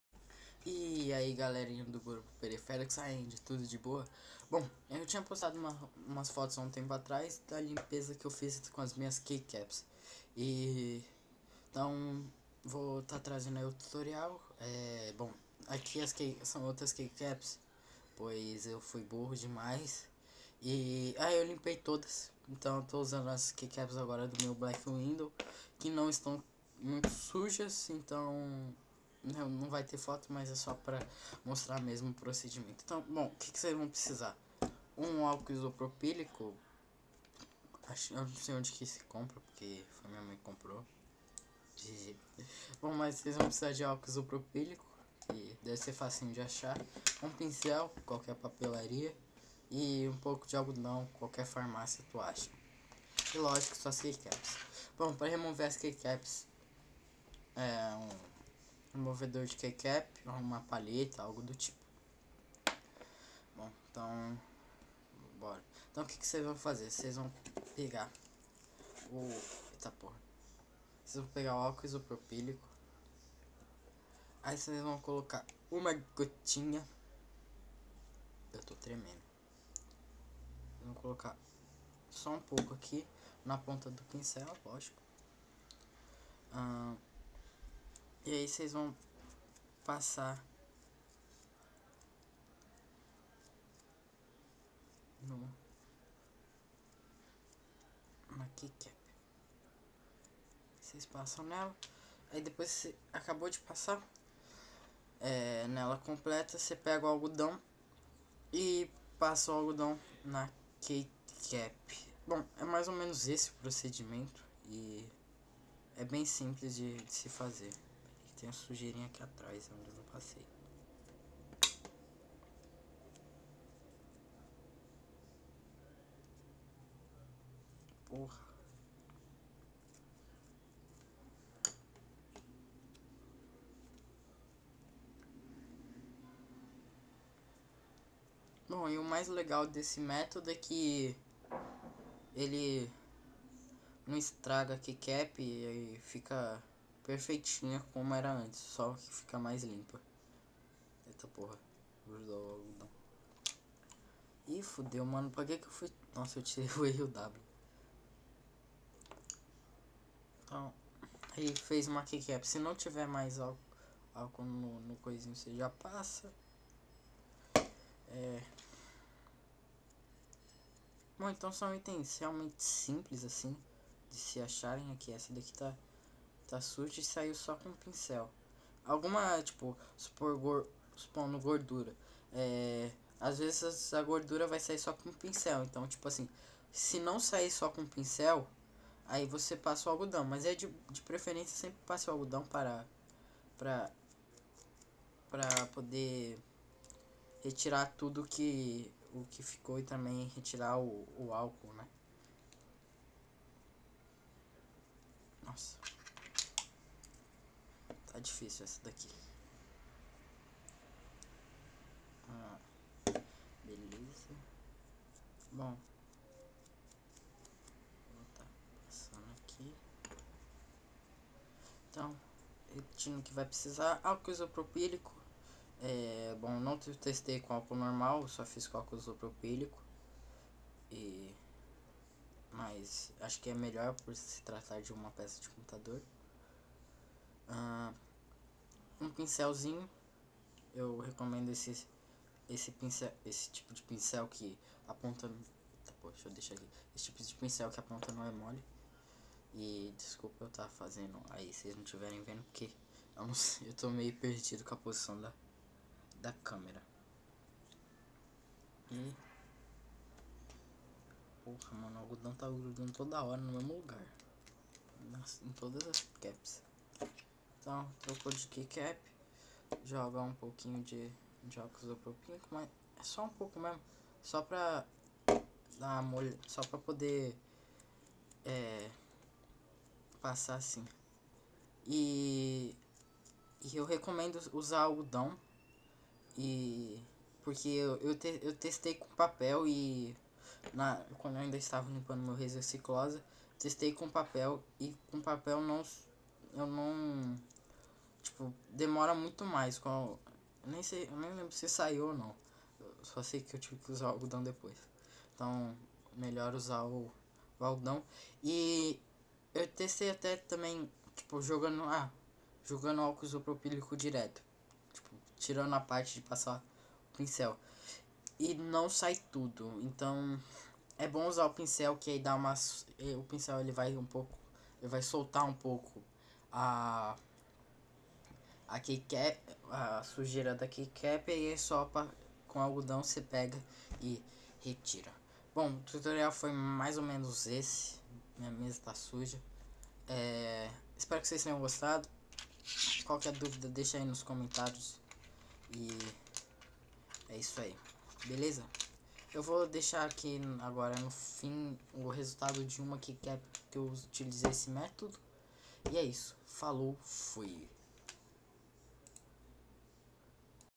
Tutorial de como limpar a Keycap
LEIA IMPORTANTE

Acabei esquecendo de por as imagens mais ok

Eai galera ,blz to trazendo um tutorial bem simples de como limpar suas KeyCaps(teclas do tecladado) ,metodo desenvolvido por min enquanto limpava meu razer cyclosa ,as Keycaps que eu usei não estavam tão sujas pois meu BW é novo ,mais de pra passar o procedimento